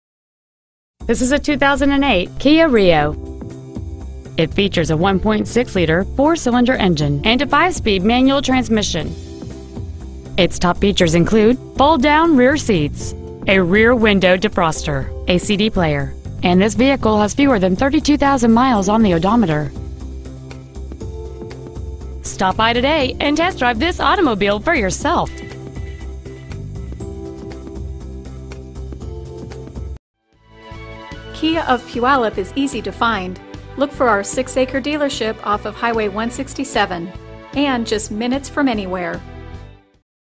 2008 Kia Rio Seattle WA
Year: 2008
 Make: Kia
 Model: Rio
 Engine: 1.6L I4 MPI DOHC
 Trans.: 5 Spd Manual
 Exterior: Light Blue
 Miles: 31,128
 Interior: Black

 2008 Kia Rio Puyallup WA